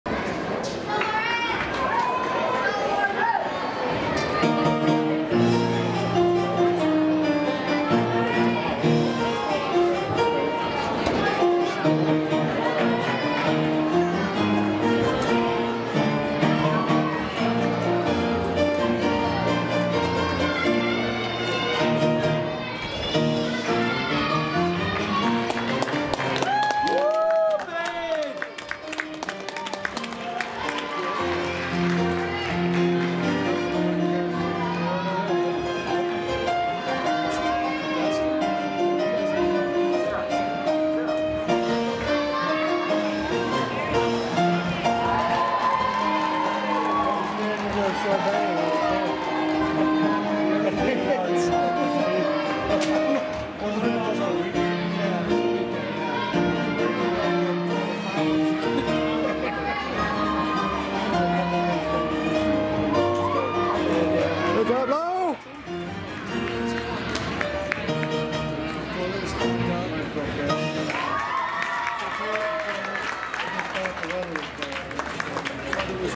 Lauren Wehner - USAG Level 4 Floor
2016 Gymnastics on the Grand Invitational
9.425 1st Place